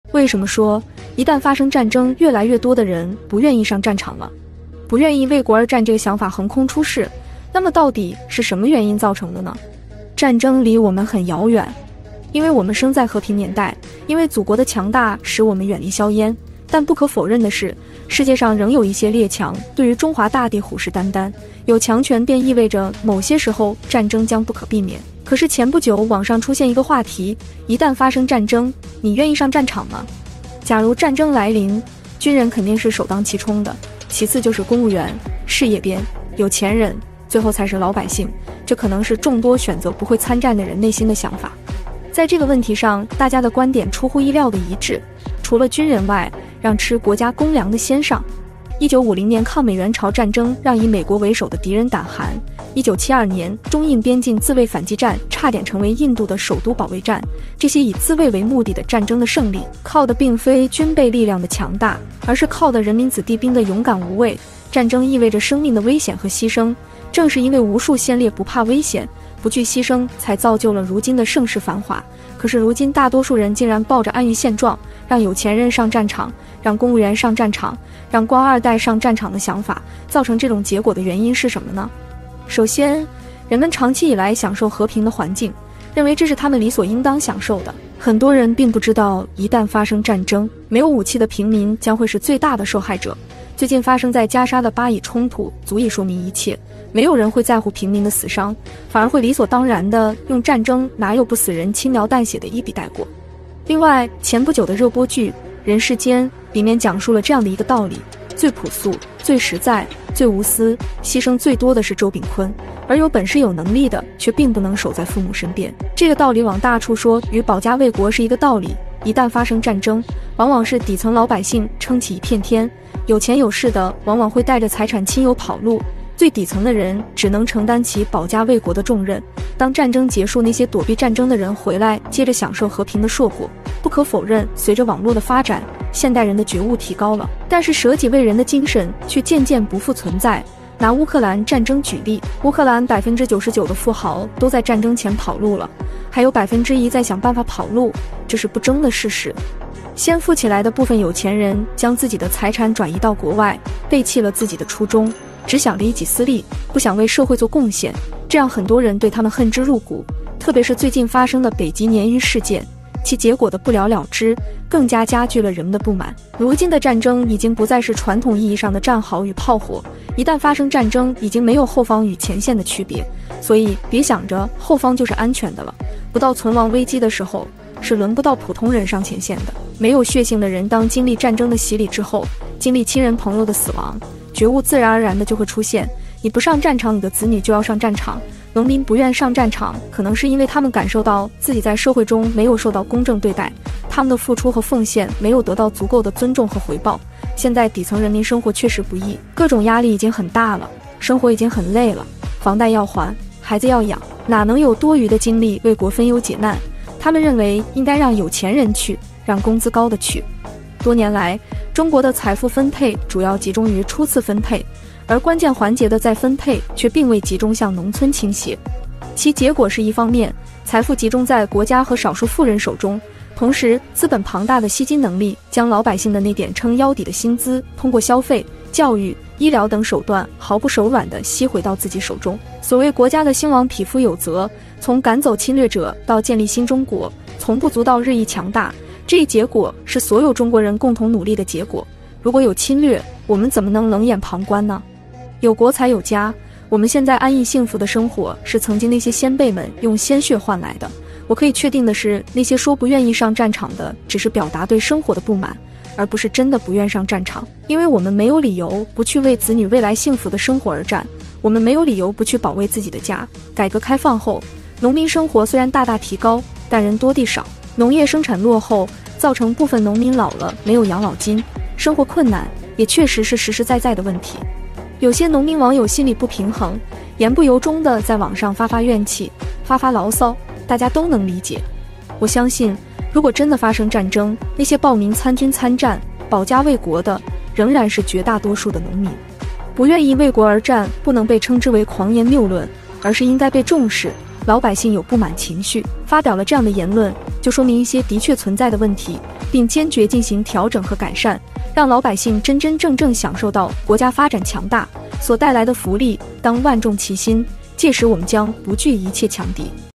为什么，越来越多的人不愿意为国而战？
... ... 让老百姓真真正正享受到国家发展所带来的福利，当万众齐心，届时我们将不惧一切强敌。
（用正能量唤醒内心深处的力量）